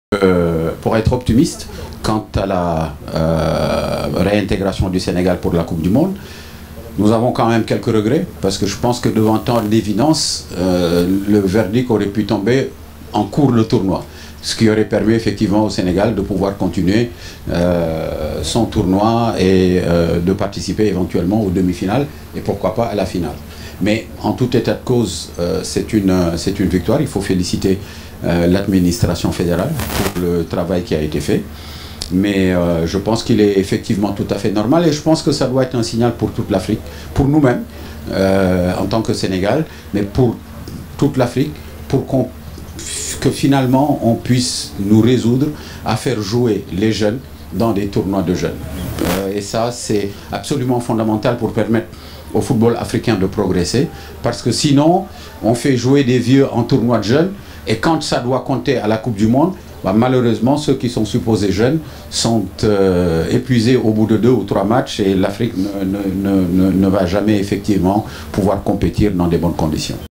Saer Seck sur la disqualification de la Guinée : «Nous avons quelques regrets...»
Selon la décision du jury de la CAF en réunion en Égypte, le Syli cadet a été disqualifié de la coupe du monde FIFA U17 qui se tiendra au Brésil du 5 novembre au 27 novembre.

Suite à une fraude sur l’âge de deux joueurs guinéens, notamment, Aboubacar Conte et Tidiane Keita, le comité exécutif de la CAF a annoncé le 20 juin la disqualification de la Guinée à la Coupe du Monde au Brésil. En outre, la Guinée a été suspendue de la compétition CAN U17 des éditions 2019 et 2021.
Voici la réaction de Saer Seck, Vice - Président de la fédération sénégalaise de Football.